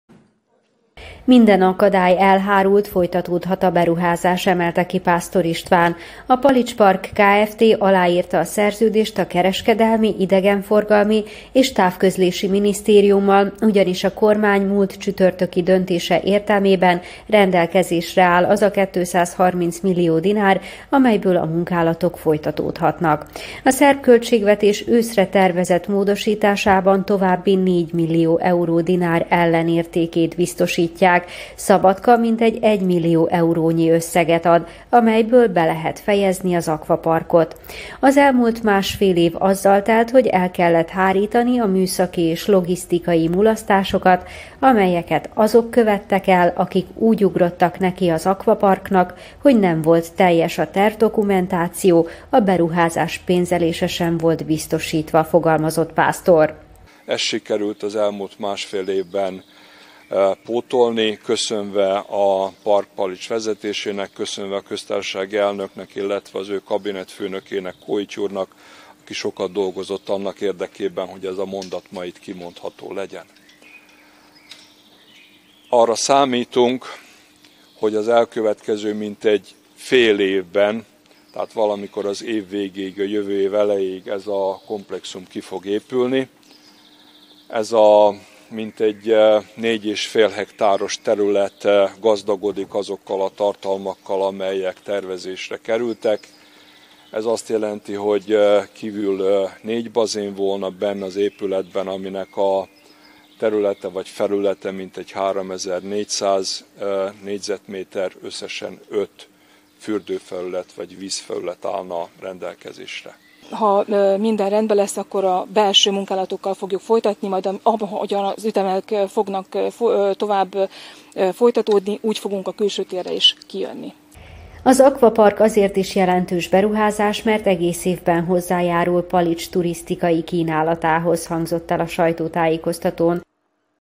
Hamarosan befejeződik a palicsi aquapark építése
Folytatódnak a palicsi aquapark kiépítési munkálatai, ugyanis az ehhez szükséges feltételeket sikerült biztosítani – jelentette be Pásztor István, a Tartományi Képviselőház és a Palics Park Kft. közgyűlési elnöke. A komplexum várhatóan fél éven belül elkészül.